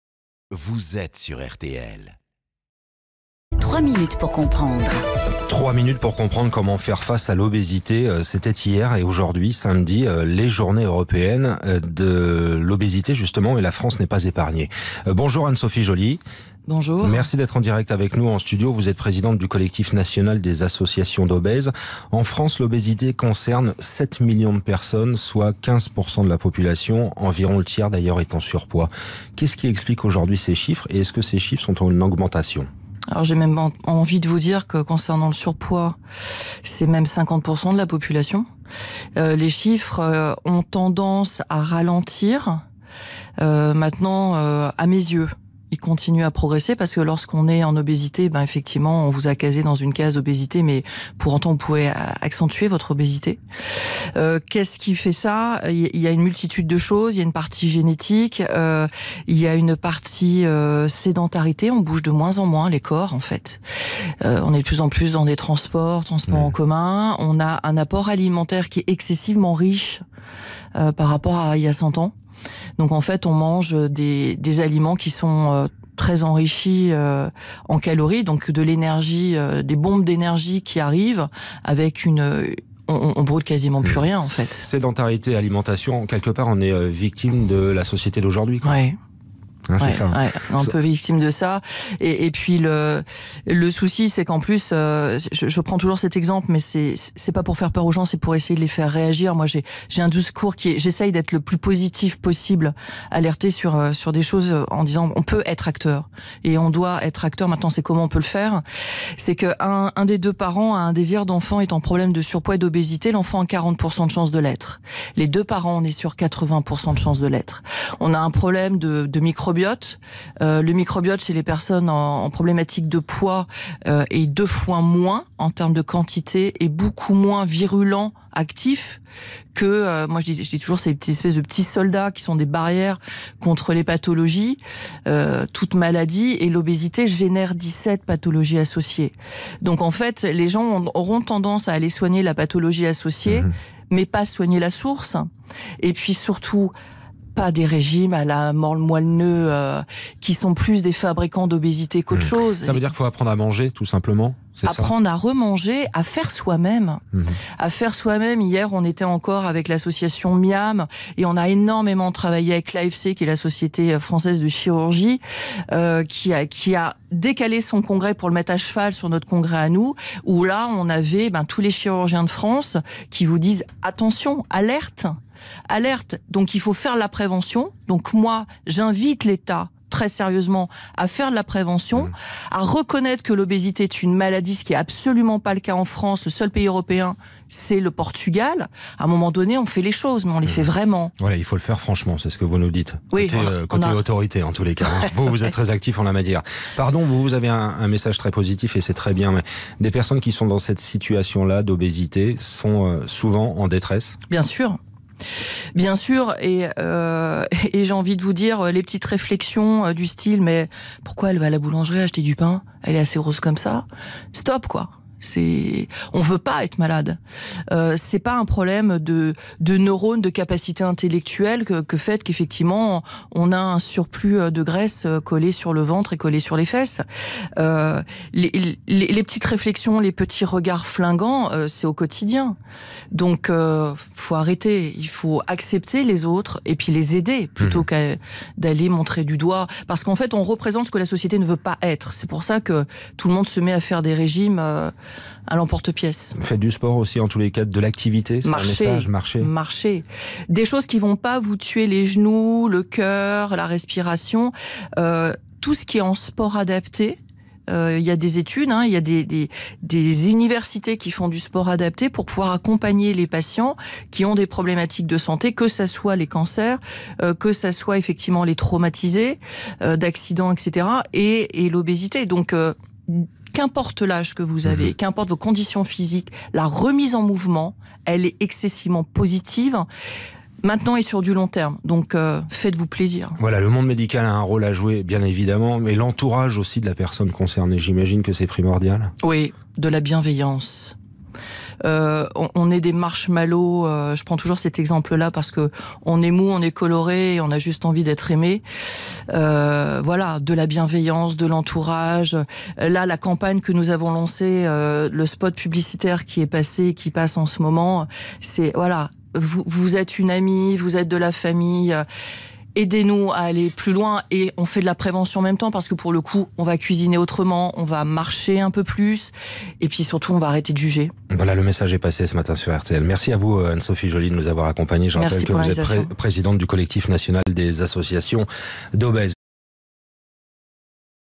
Journée de l'obésité : comment expliquer l'évolution de cette maladie ?
INVITÉE RTL - Anne-Sophie Joly, présidente du collectif national des associations d'obèses. En France, l'obésité concerne 7 millions de personnes.